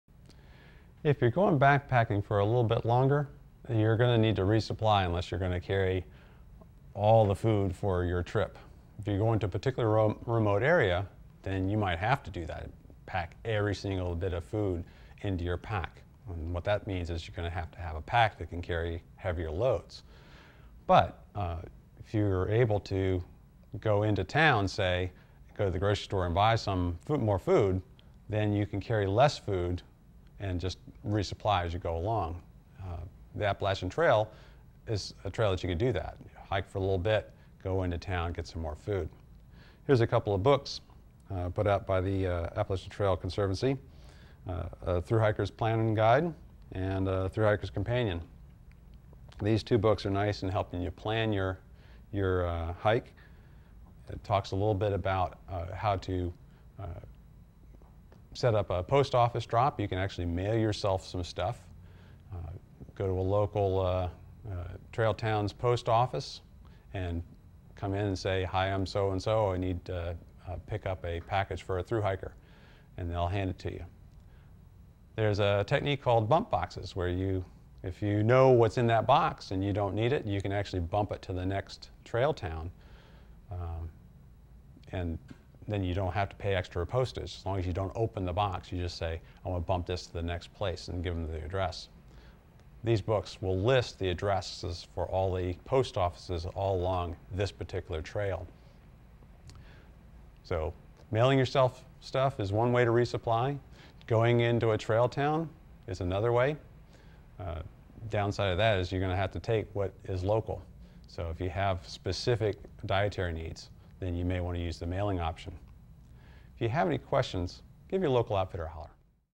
Camping & Backpacking Tips & Gear : Camping Trip Resupply Tips
For longer camping trips you may need to resupply your food at some point on the trail.  Learn more about camping resupply with tips from an outdoor activity expert in this free camping video.

Expert: Brant Bowers
Bio: Brant Bowers has worked in the Camping Department at Bill Jackson's sporting goods store since 2004.